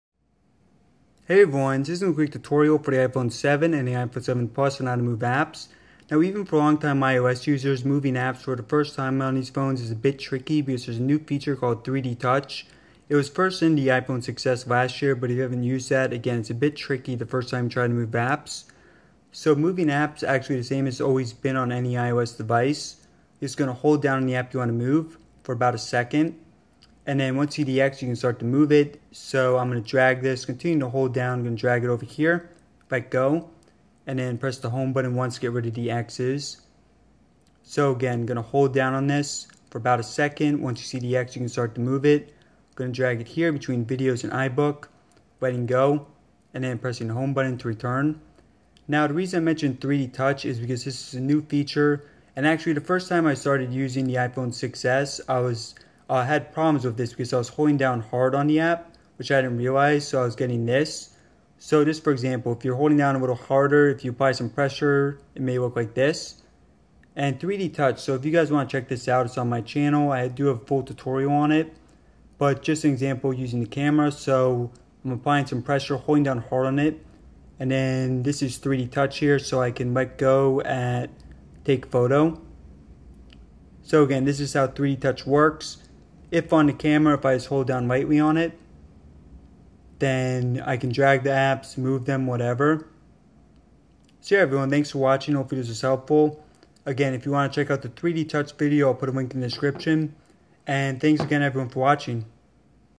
iPhone 7 How to Move Apps Icons (iOS 10)
Apple iPhone 7 and 7 Plus how to move apps and rearrange on iOS 10. Icons can be dragged by holding down lightly.